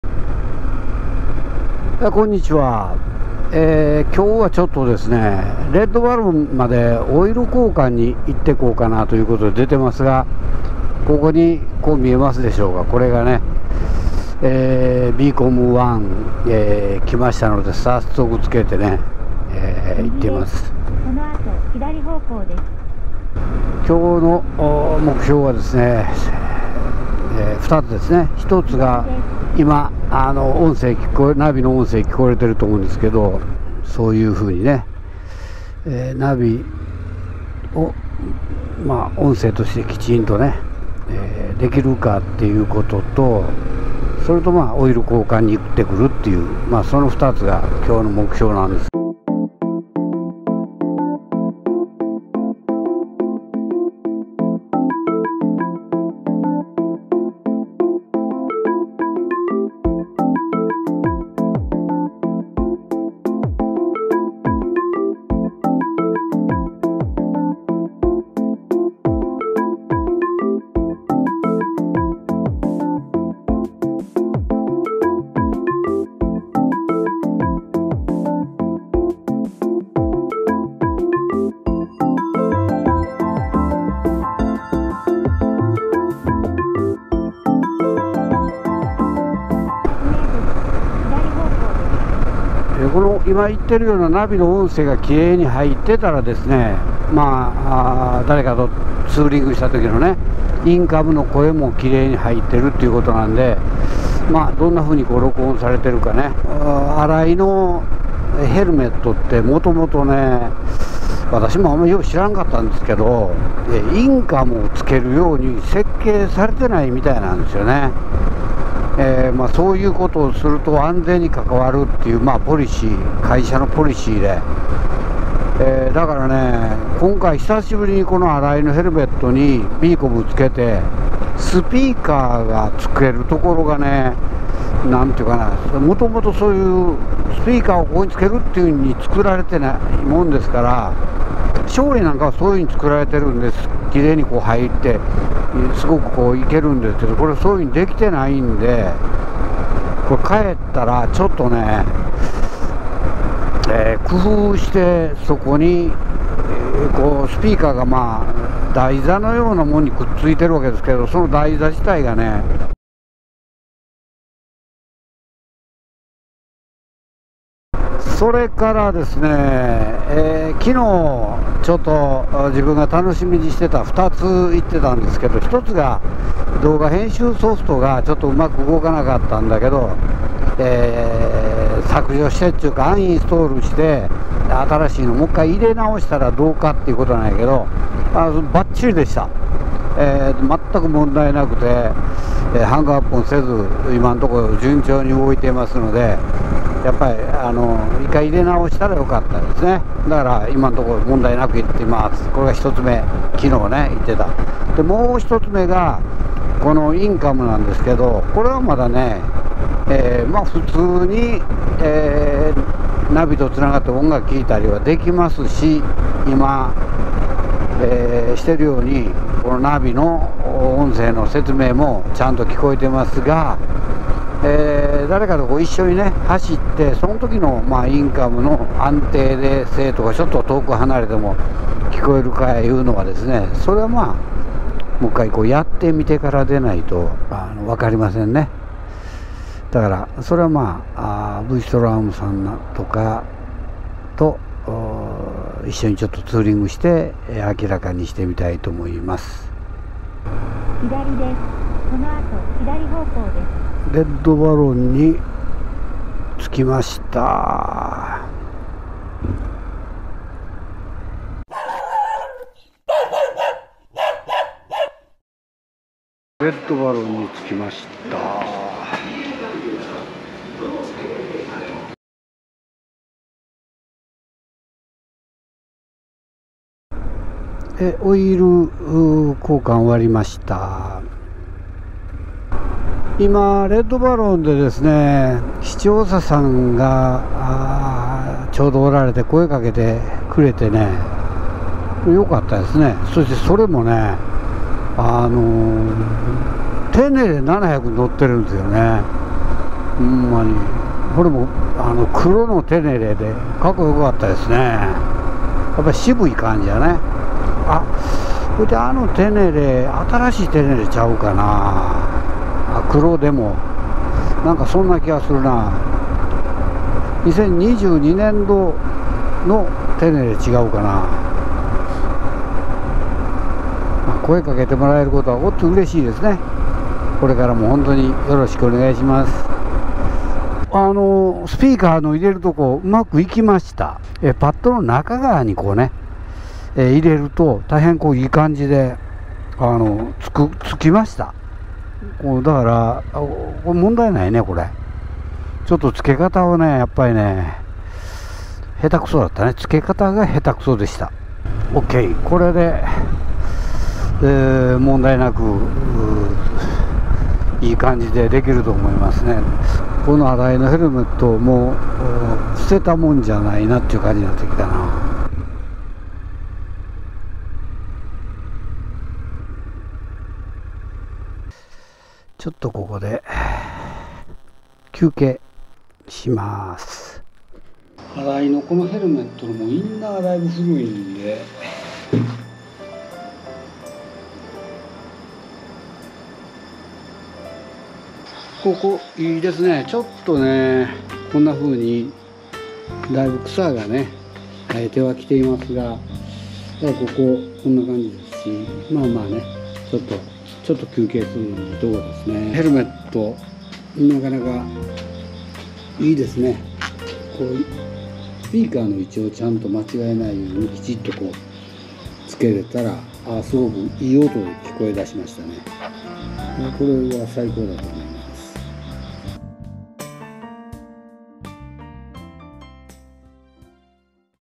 第702話 【テネレ700】レッドバロンでオイル交換
オイル交換にレッドバロンに行ったら、そこで視聴者さんと会った。楽しい時間を過ごすことができた。
I went to Red Baron for an oil change and met a viewer there. We had a good time.